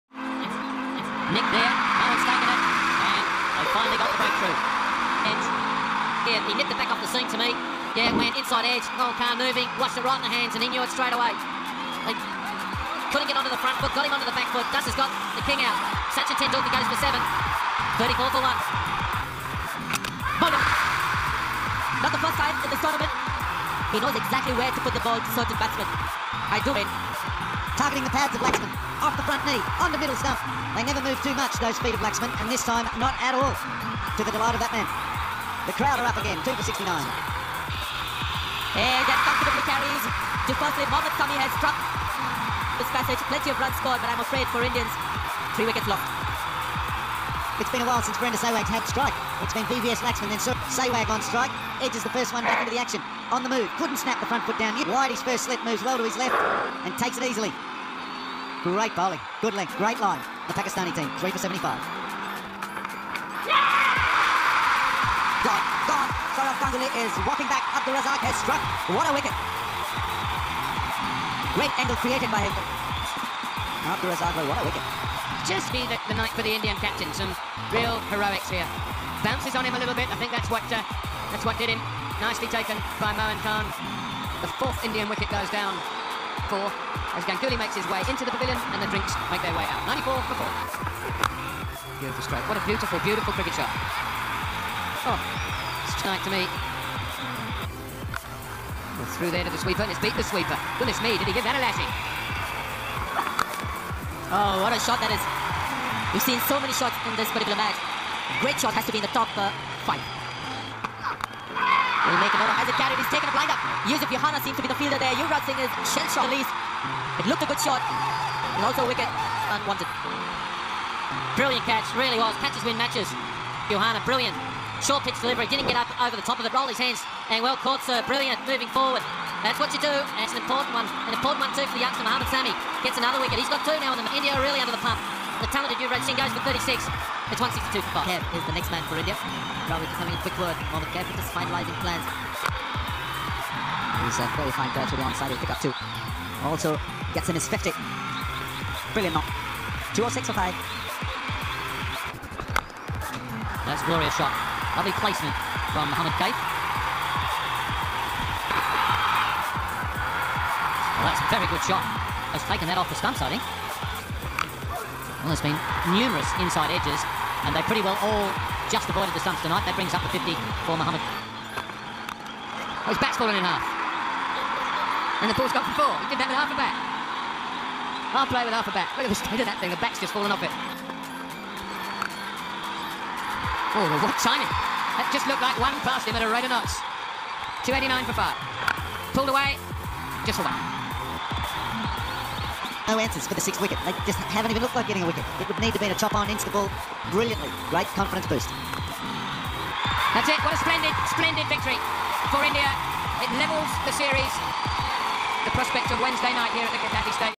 🔥India Vs Pakistan 2004 | India were 94/4 then DRAVID YUVRAJ KAIF made India comeback | Ind vs Pak💥🔥
🔥India Vs Pakistan 2004 | India were 94/4 then DRAVID YUVRAJ KAIF made India comeback | Ind vs P💥🔥

Watch Most Thriller Matches :-